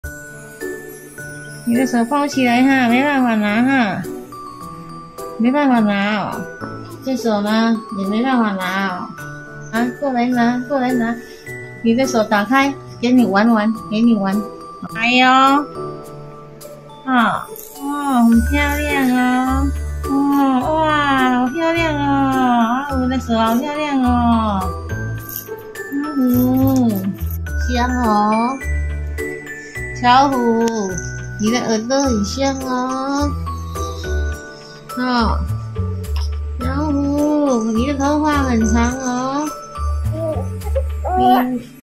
阿嬤給寶寶戴手環，玩得很開心【2個月大】2 Months Old
Hello, it's baby's Vlog.
We update everyday.
Thank u for watching.
每天更新寶寶成長日記
(我們預計拍到滿週歲為止)